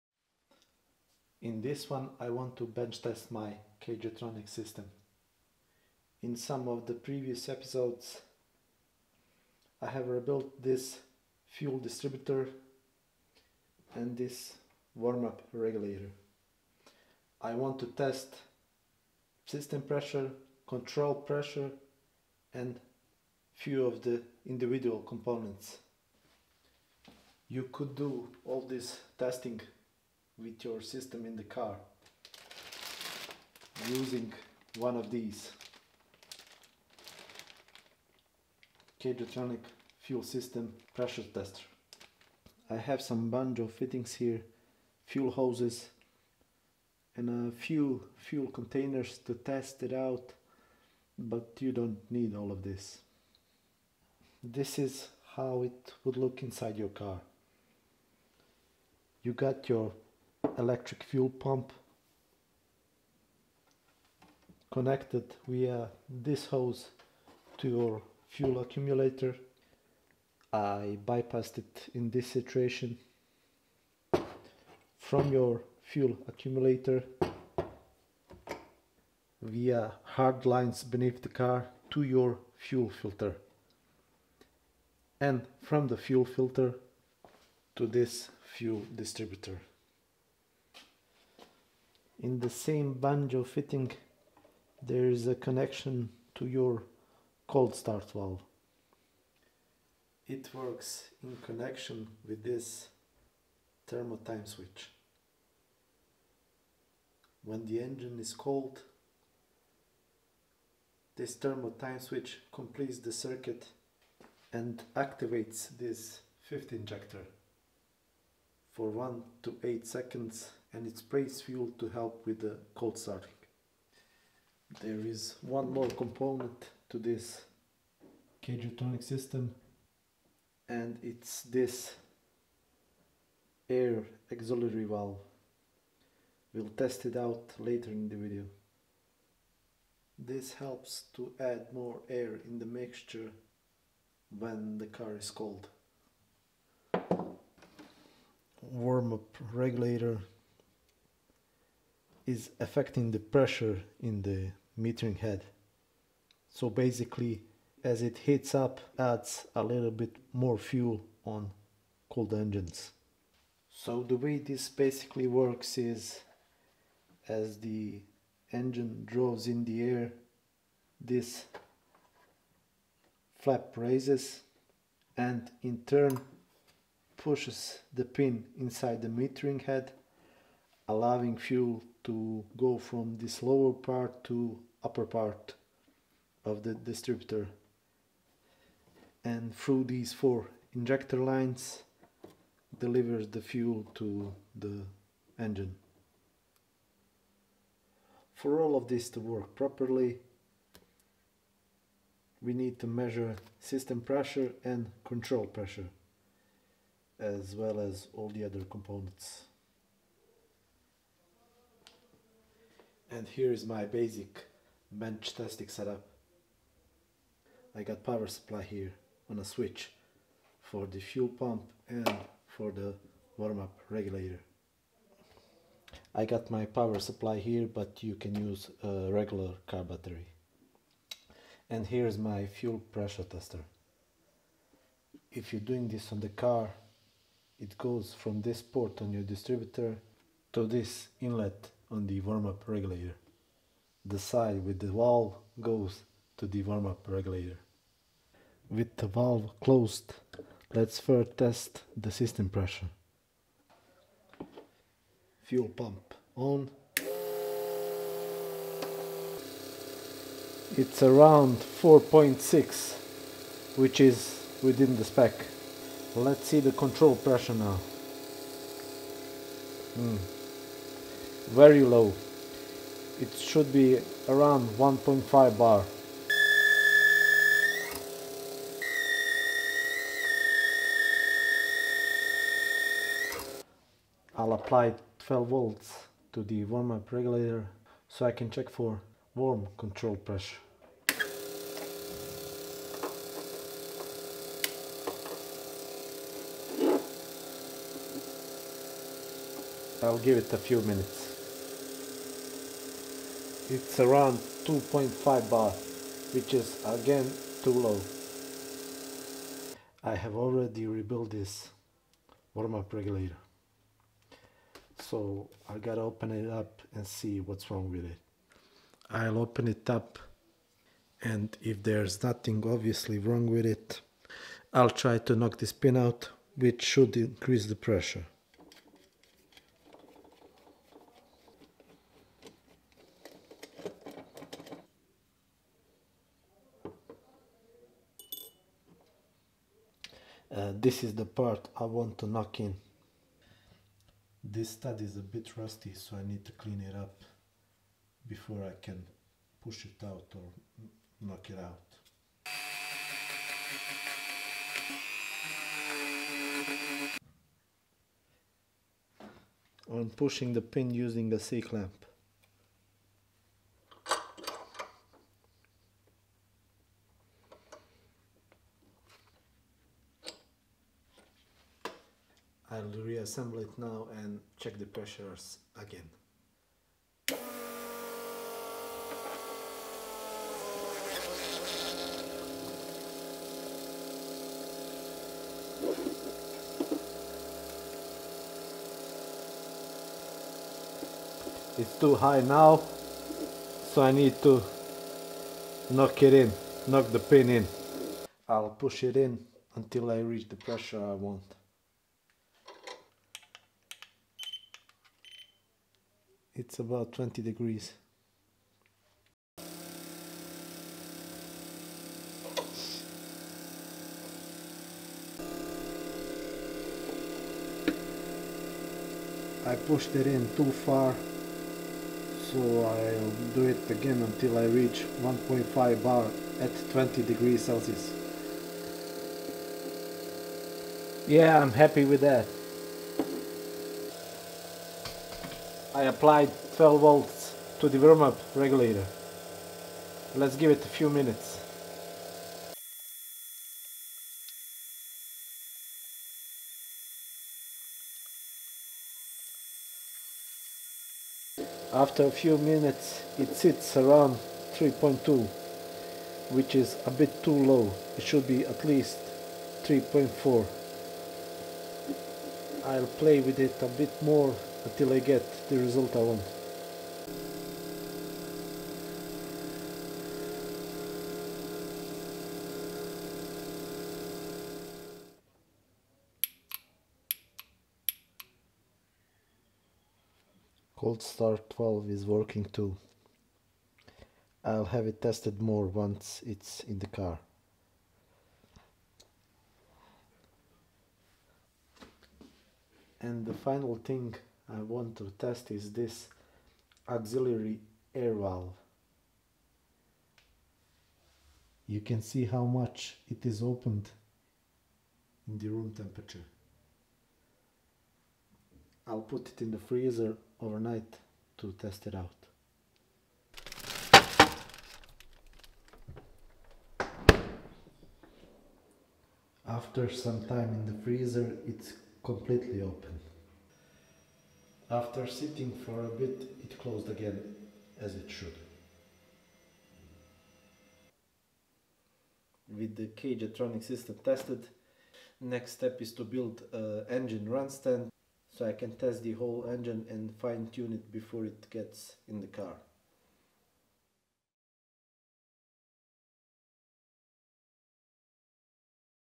Bosch K-Jetronik CIS System Bench Test
Welcome to Restoration Apprentice!

Testing my rebuilt warm up regulator (WUR) and fuel distributor in this one.

Armin